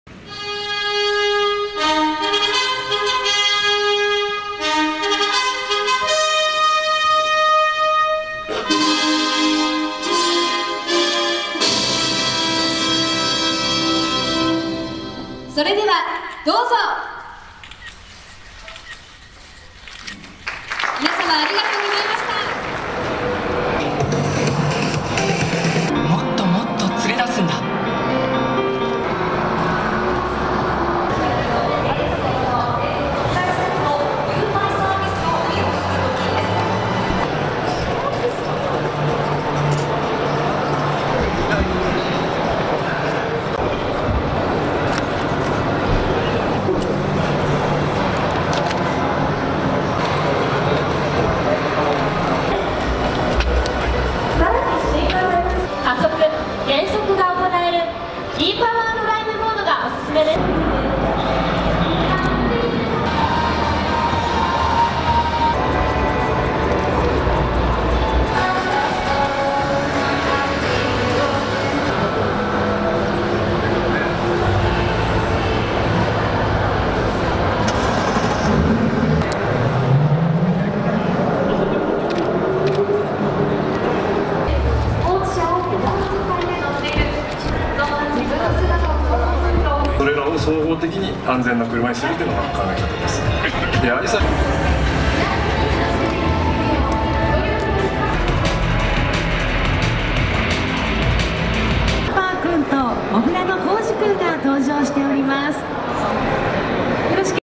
「大阪モーターショー2017」
インテックス大阪で12月8日、「第10回 大阪モーターショー」が始まりました。今回は国産車、国産二輪車、輸入車、輸入二輪車など合計32ブランドが出展。ステージイベントも展開し、9日にレーシングドライバーの佐藤琢磨選手、10日にお笑いタレントの岡田圭右さん（ますだおかだ）がスペシャルゲストで登場します。